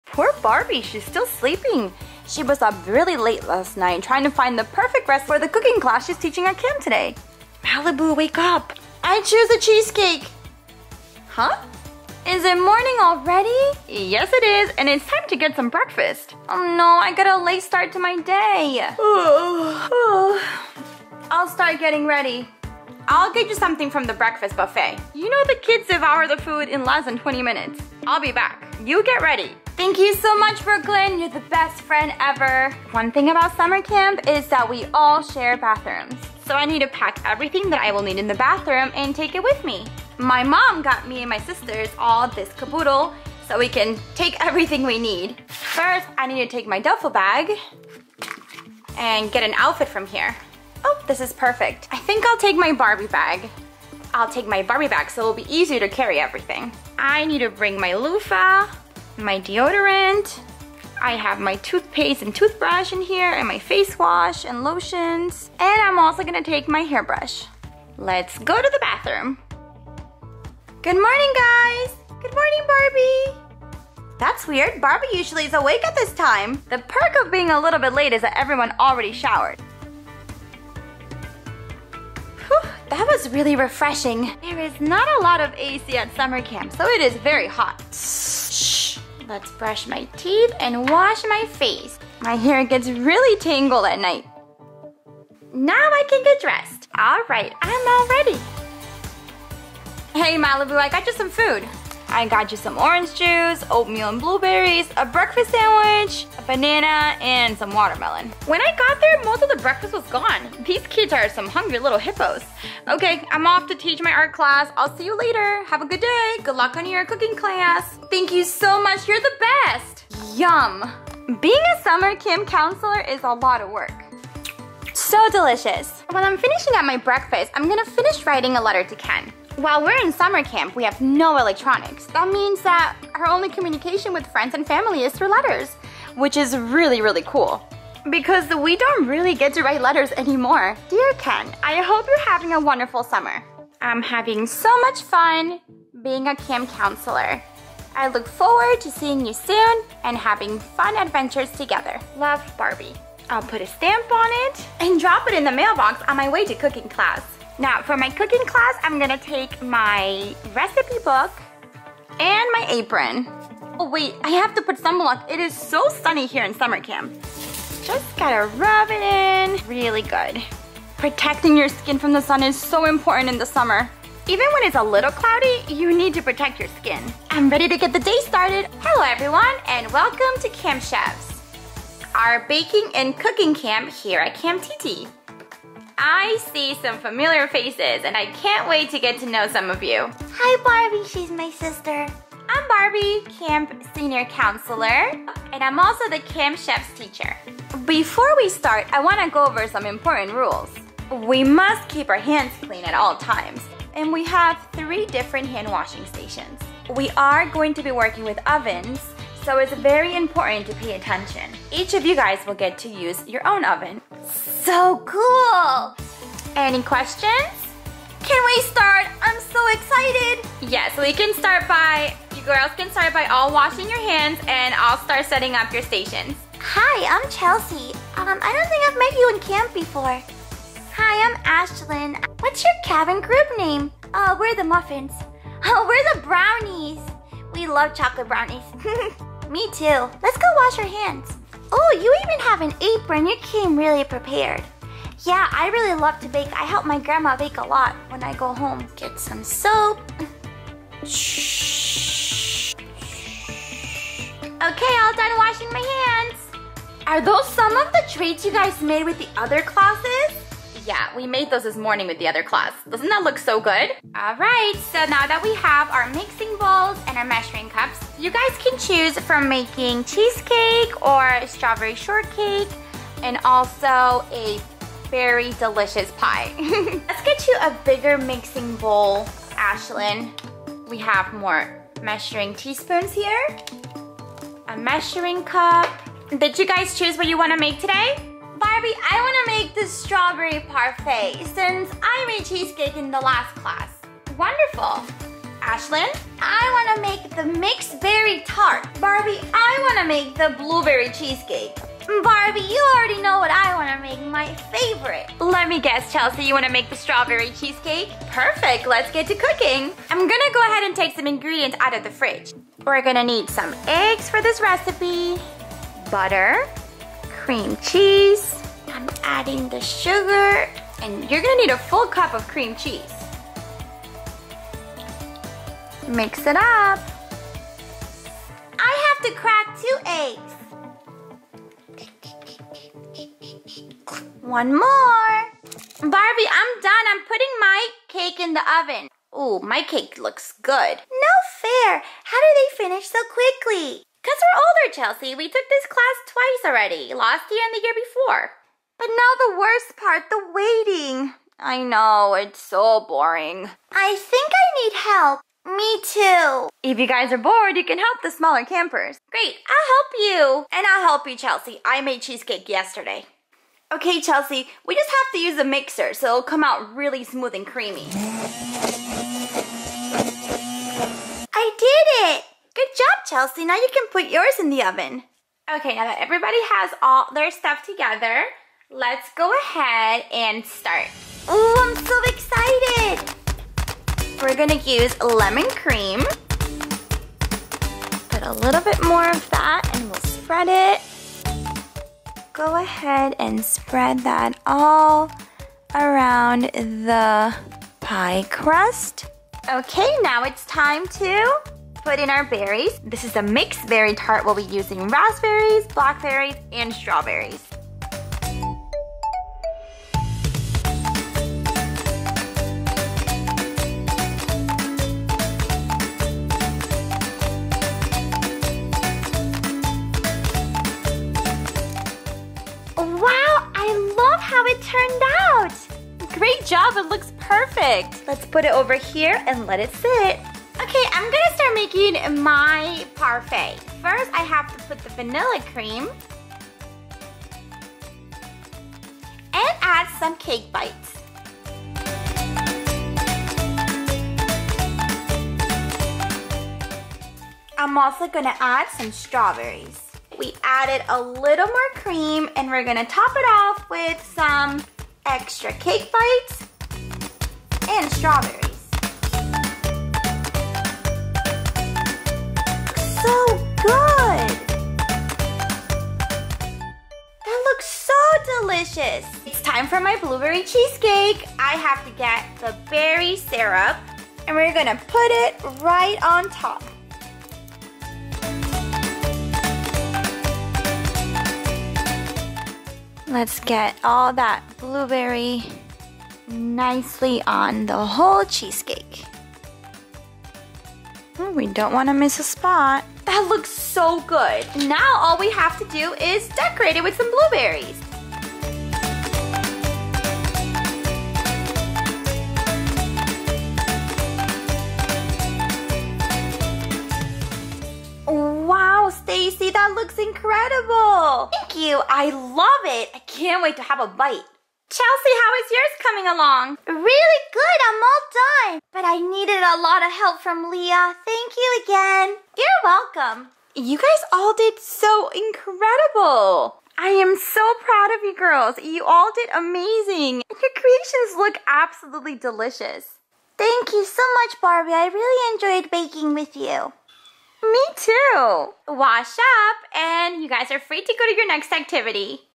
Barbie Summer Camp Counselor - Day in The Life
Barbie Summer camp counselor day in the life. Barbie and Brooklyn get ready for a full day of fun in summer camp. They wake up in their cabin and get breakfast.Barbie is sleeping inside a cabin. Barbie gets ready to teach the camper kids how to bake. We make miniature food with miniverse make it mini. The kids are so excited to make their own food at camp and feel so proud of how it turned out. they learned to be patient and work as a team.